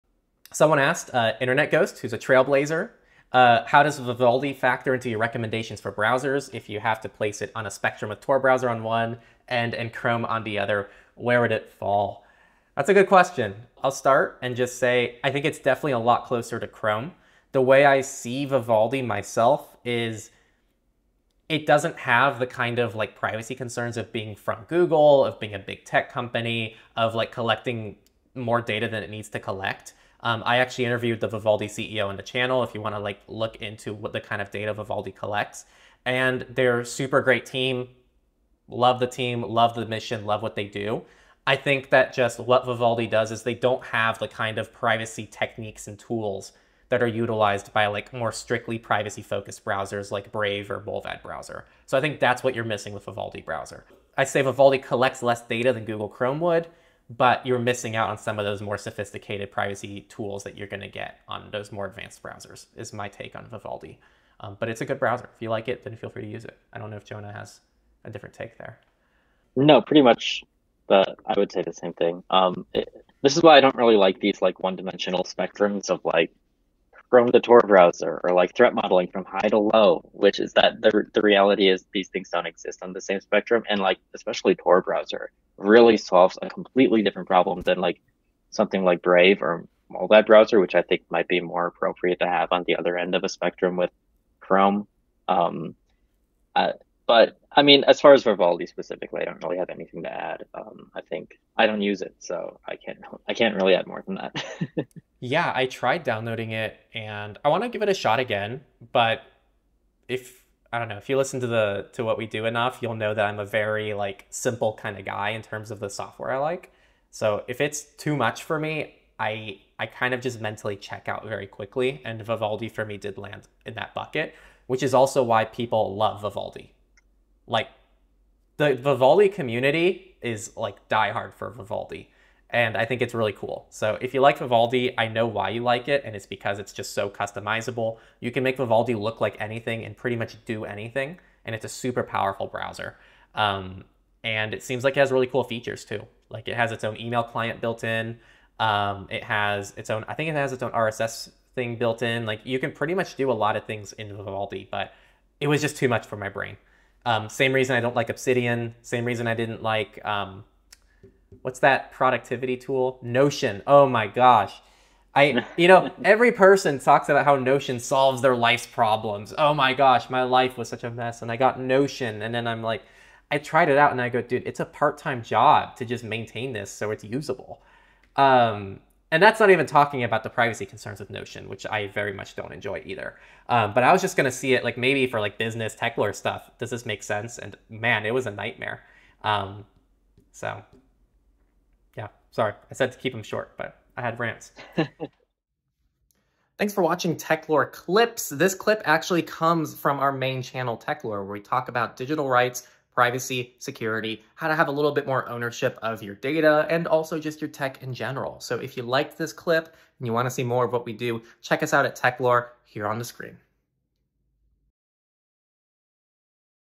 Why We Don't Recommend Vivaldi for Privacy
Looking for a customizable browser that prioritizes privacy? Vivaldi offers a unique blend of features and a focus on user control. In this video, we'll explore Vivaldi's privacy settings, compare it to other browsers, and discuss the pros and cons of using it.